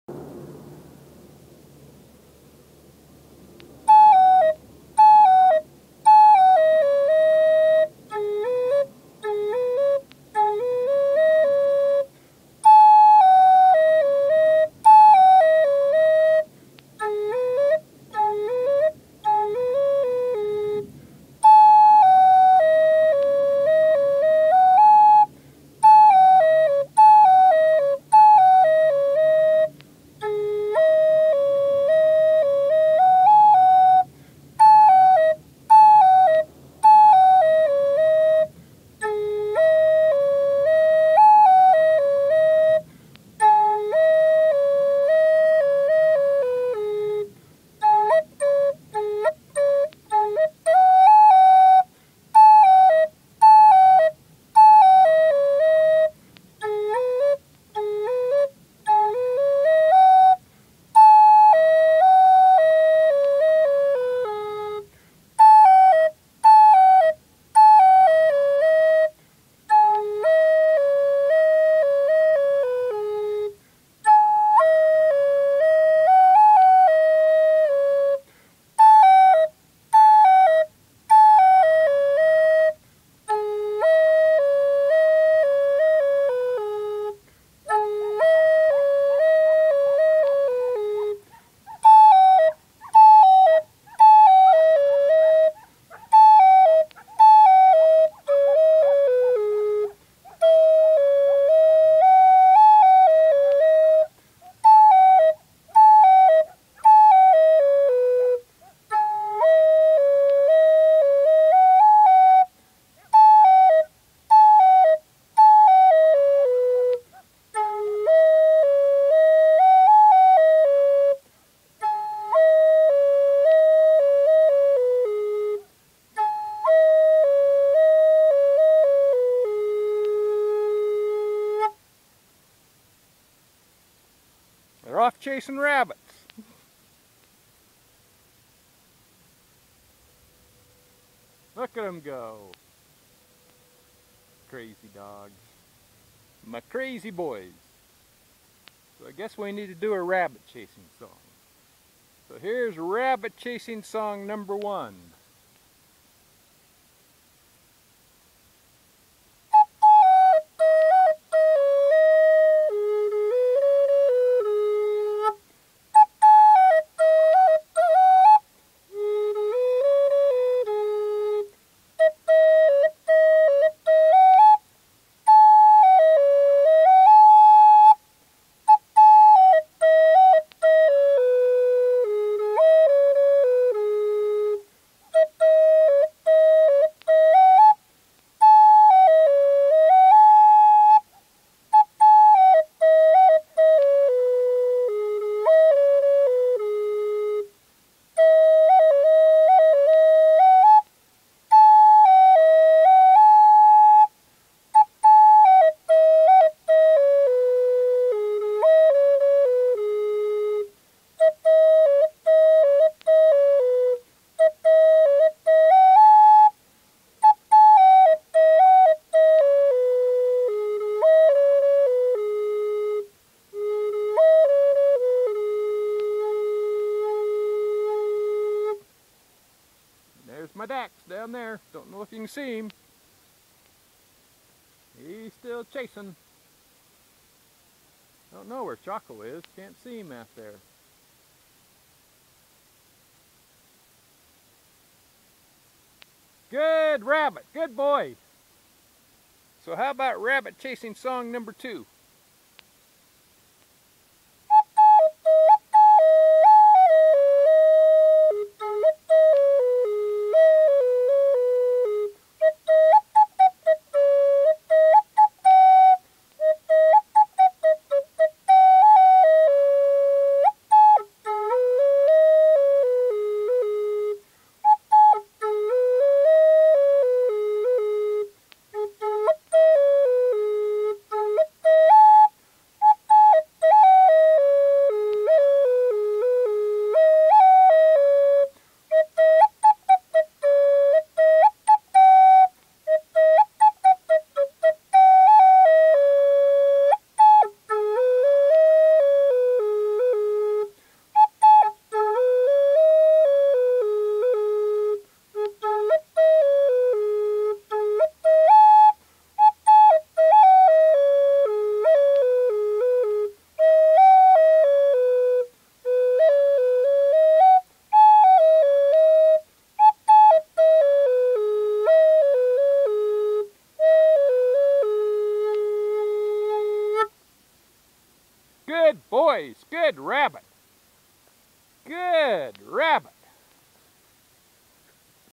3 rabbit chasing songs (with live accompaniment)
It was a beautiful morning, and I was trying a new song I'm working on. about 2/3's of the way through it, my dogs took off after a rabbit (you can hear their yipping and see me change my attention to watch them). That's the "live accompaniment". Well, it always takes a while for them to come back, so I also play rabbit chasing song #1 and #2. Hope you don't get bored!

Recorded on an iPod nano on 1/7/10.